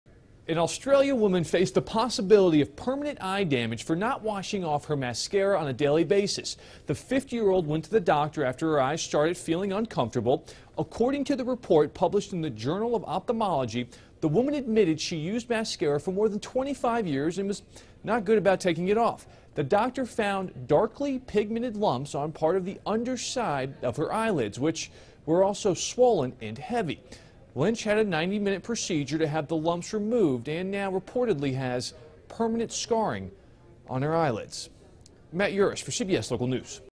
Woman Reportedly Leaves Mascara On Too Long, Almost Loses Her Eyesight
A woman in Australia who had a habit of not washing off her mascara developed serious eye problems that could have taken her vision, according to a new report. Matt Yurus reports.